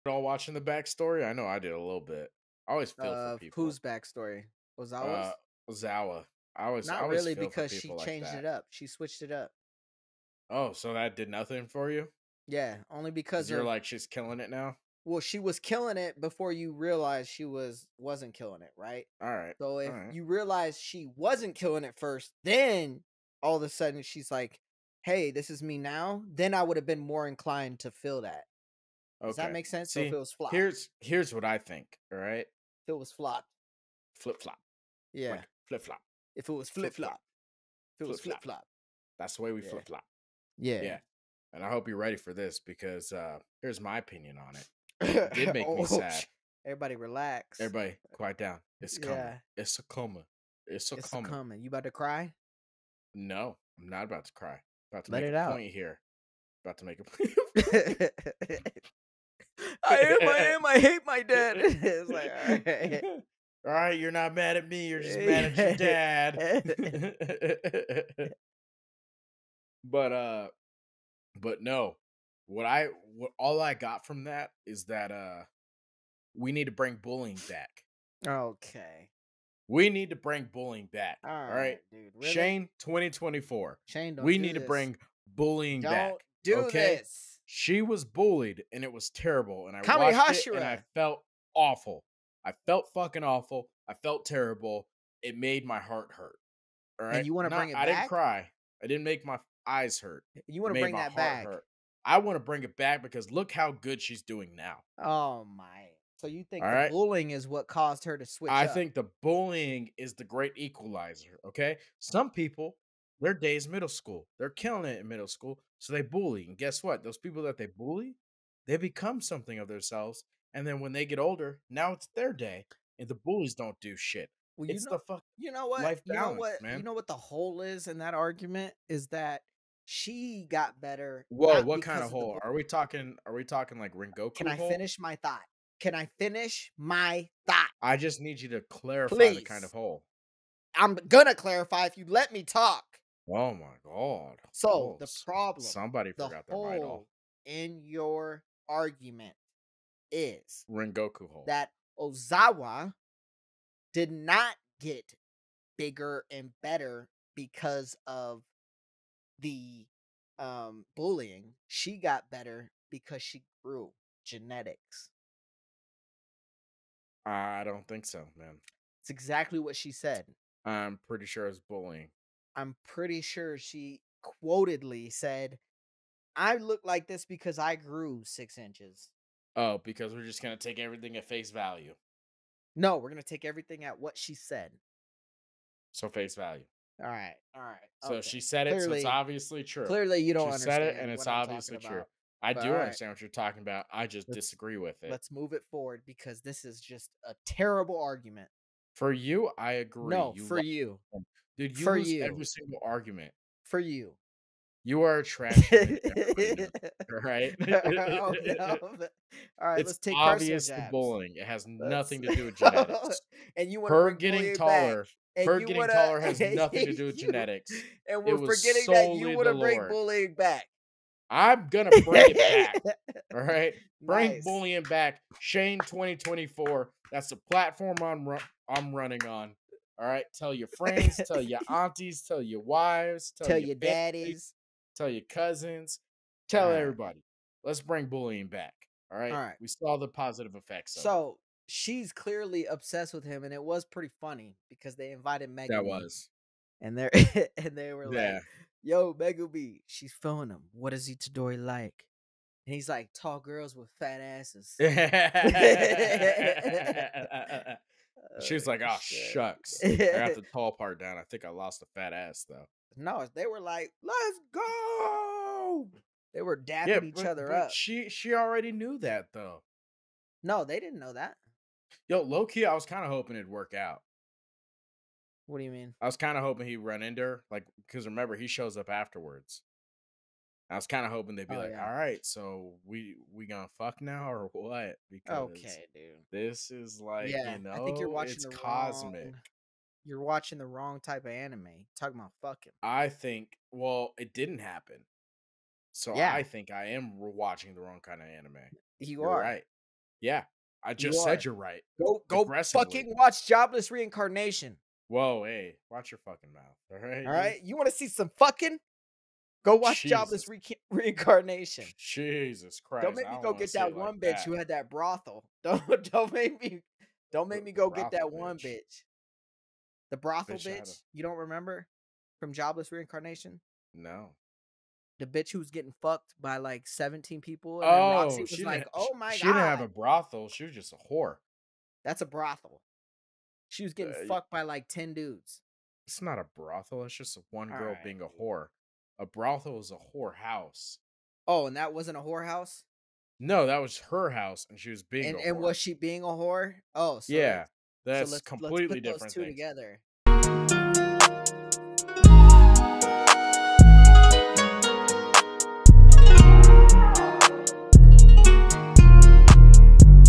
Did BULLYING Change Yuko Ozawa?! | JJK Shibuya Incident Review | OOP Episode 15 Clips
Instrumental By Timmy The Destroyer